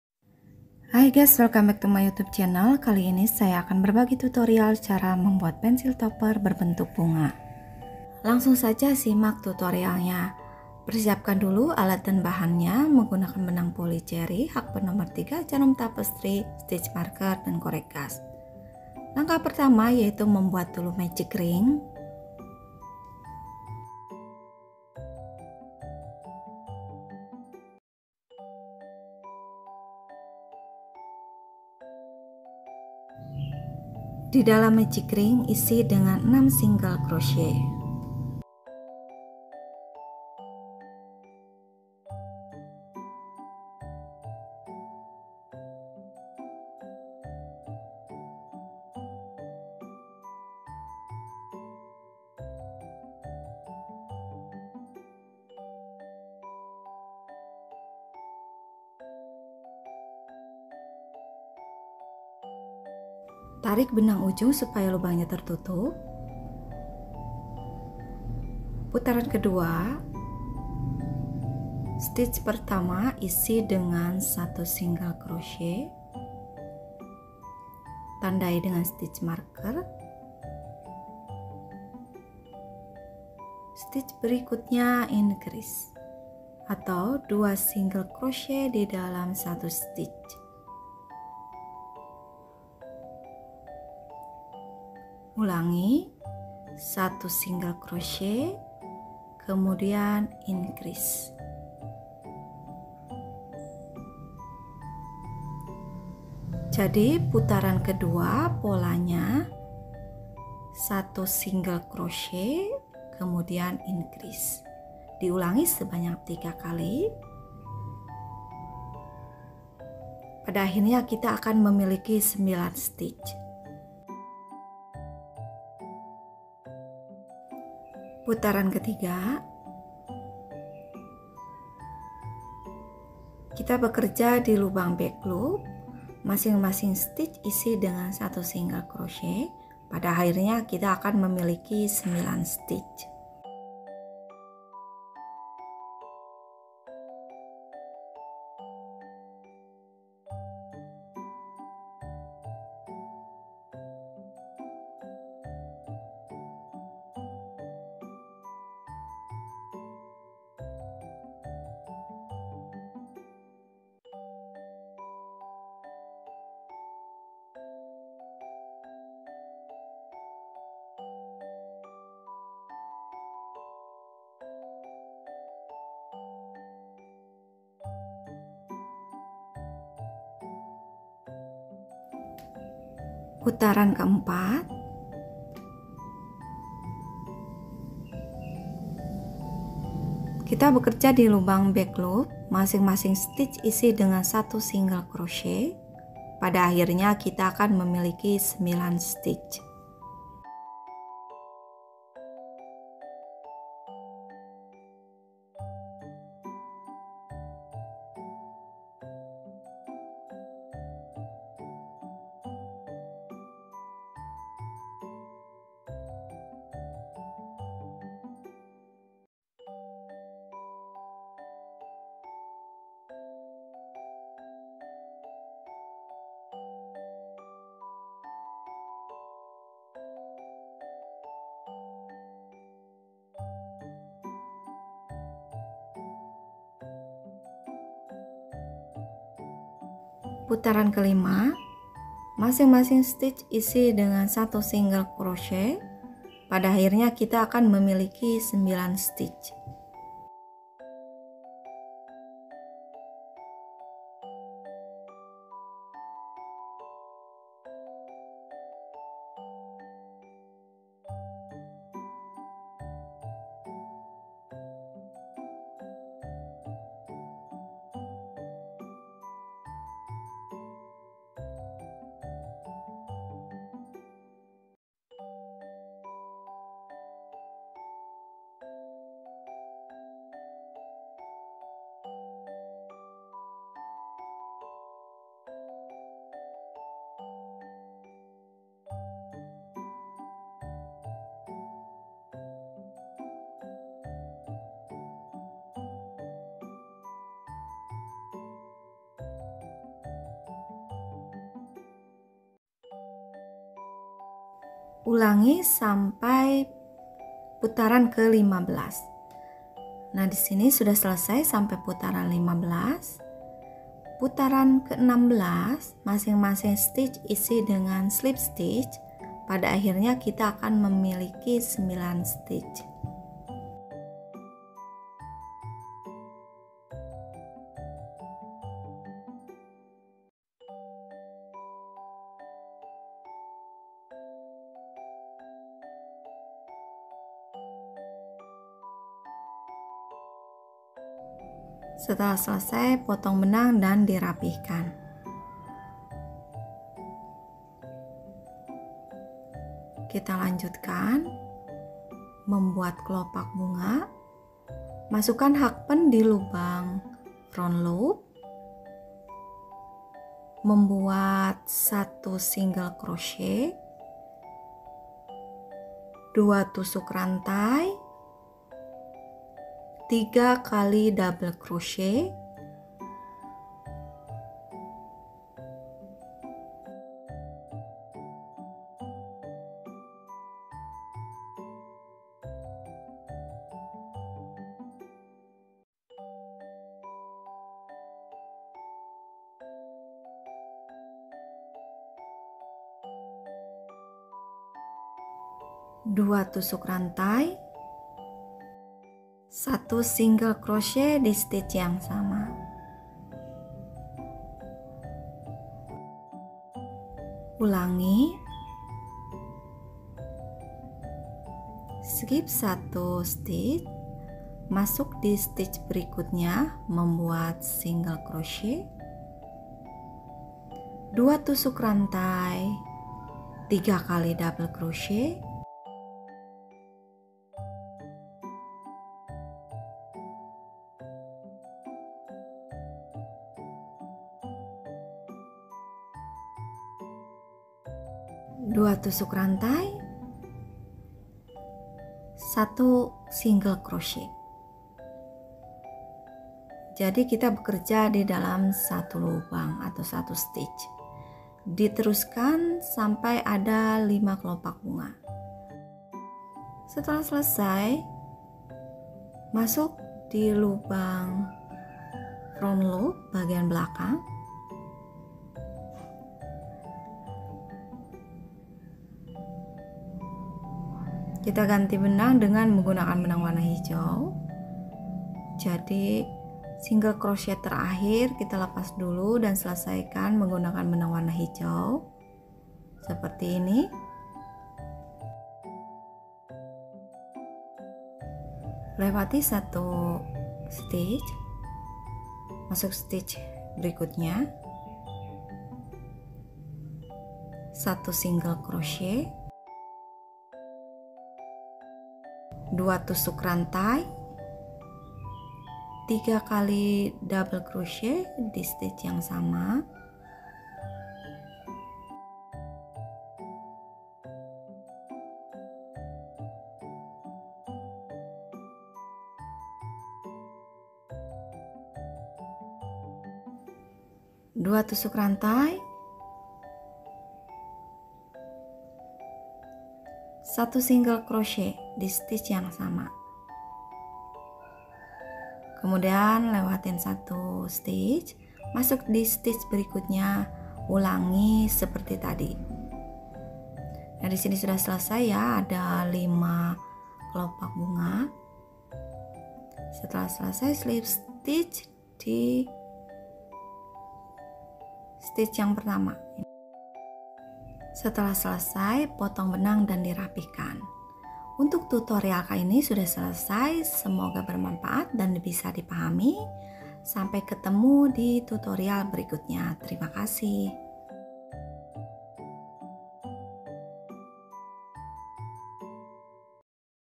Crochet flower pencil topper | hiasan pensil rajutan
Alat dan bahan yang digunakan adalah :
1. Benang polycherry
2. Crochet hook / Hakpen size 3/0 atau 2.20mm
3. Jarum sulam / tapestry needle
4. Gunting / lighter